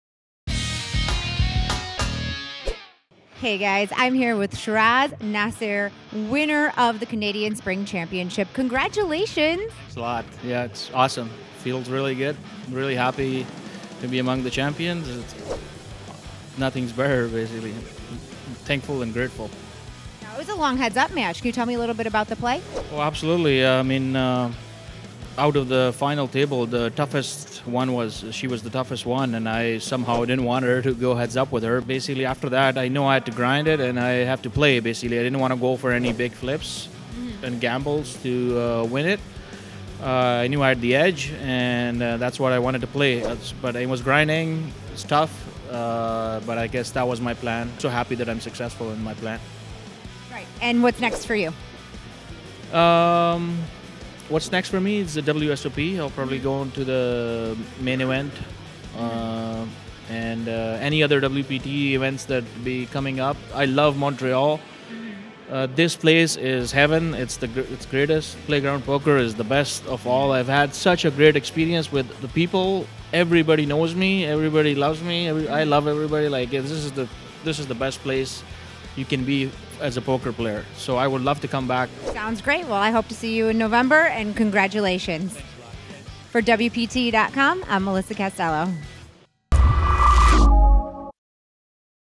Season XIV partypoker WPT Canadian Spring Championship: Sheraz Nasir Winner Interview
Sheraz Nasir is our first champion of Season XIV taking down the partypoker WPT Canadian Spring Championship at Playground Poker Club.